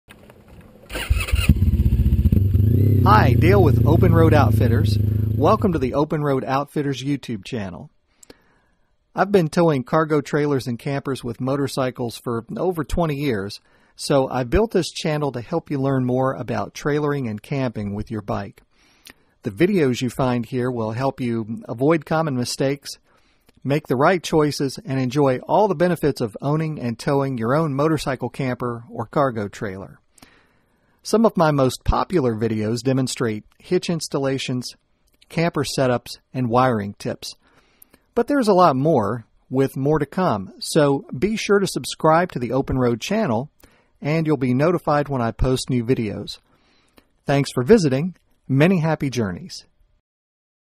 Welcome to Open Road Outfitters Channel
Audio transcript:

Hi, Dale with Open Road Outfitters here. Welcome to the Open Road Outfitters YouTube channel.

I've been towing cargo trailers and campers with motorcycles for over twenty years, so I built this channel to help you learn more about trailering and camping with your bike. The videos you find here will help you avoid common mistakes, make the right choices and enjoy all the benefits of owning and towing your own motorcycle camper or cargo trailer.

Some of my most popular videos demonstrate hitch installations, camper setups and wiring tips. But there is much more, with more to come.

Thanks for visiting and many happy journeys!